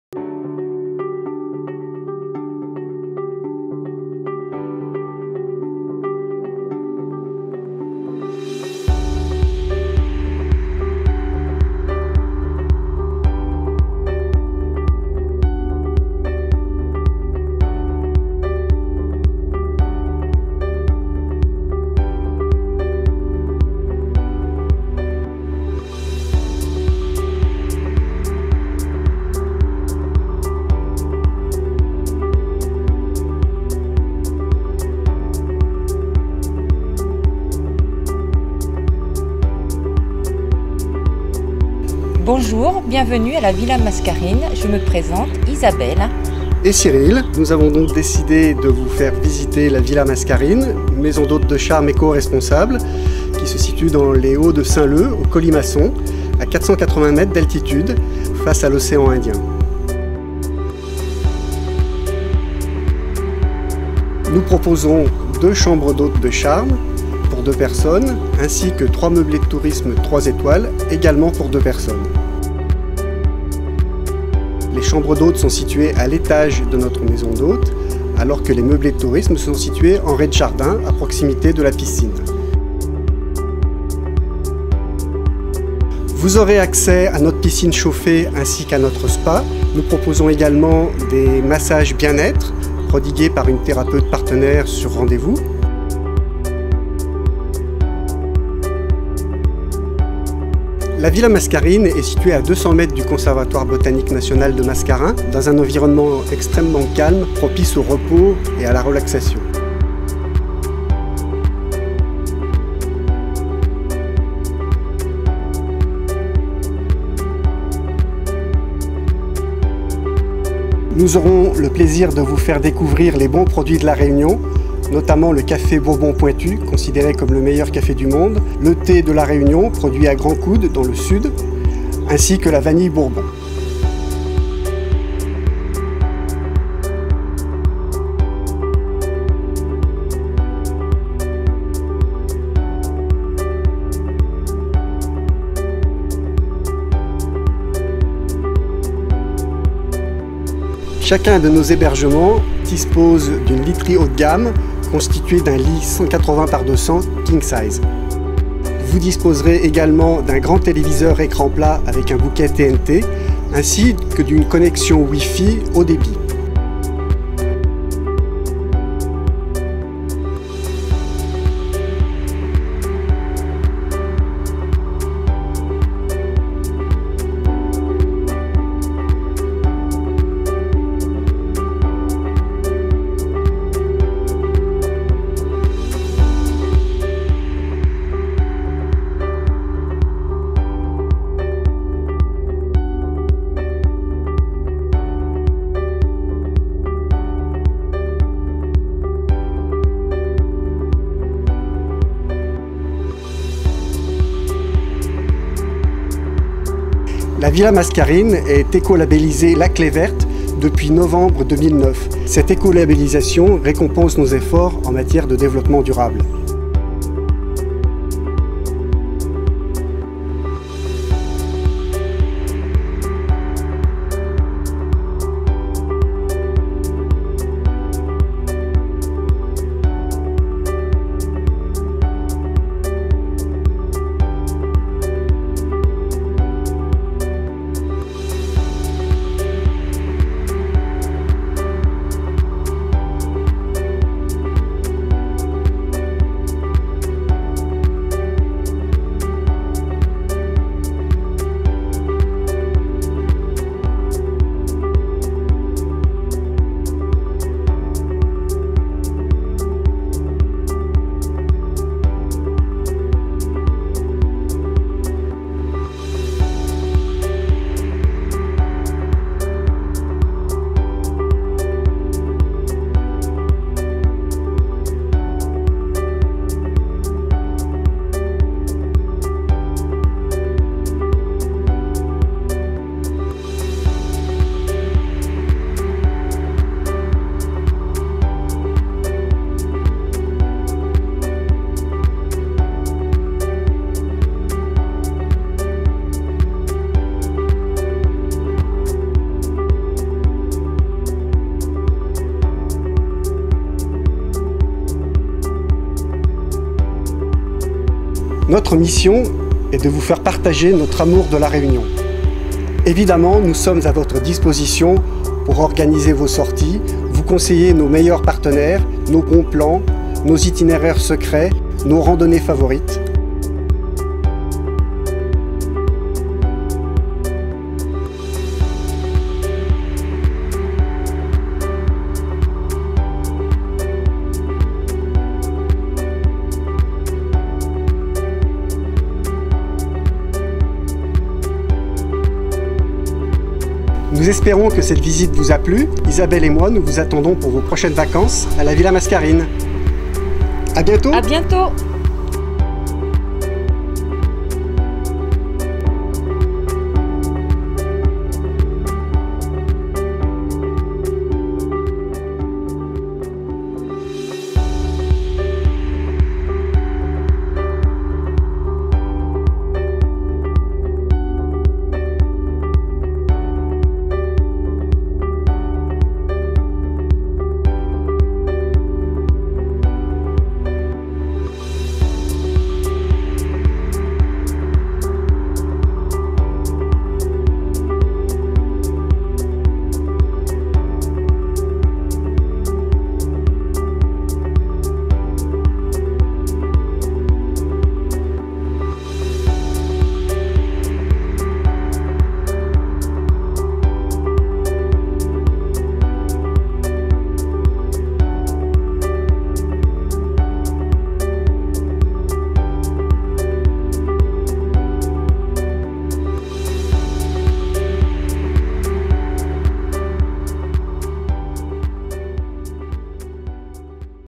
VISITE ET PRÉSENTATION, MAISON D' HÔTES DE CHARME VILLA MASCARINE A SAINT LEU
Visite et présentation de la Villa Mascarine, maison d’hôtes de charme écoresponsable, Saint Leu, Île de la Réunion.
Isabelle et Cyrille sont heureux de vous accueillir chez eux dans leurs chambres d’hôtes et leurs gîtes, lors de vos vacances sur l’île de la Réunion, dont les cirques, pitons et remparts sont classés au Patrimoine Mondial de l’UNESCO.Sur la côte ouest, dans un environnement exceptionnel dédié au bien-être, avec piscine chauffée et spa, la Villa Mascarine***, maison d’hôtes de charme éco-responsable, est idéalement placée pour pouvoir rayonner sur toute l’ile de la Réunion.  A 480m d’altitude, au cœur d’un jardin tropical surplombant l’Océan Indien et Saint Leu sur elle propose : 2 chambres d'hôtes de charme, pour 2 personnes et 3 meublés de tourisme classés 3 étoiles, pour 2 personnes. La Villa Mascarine*** est le premier hébergement de la Réunion écolabellisé Clef Verte.
396 rue Georges POMPIDOU - Les Colimaçons - 97436 SAINT LEU fax gsm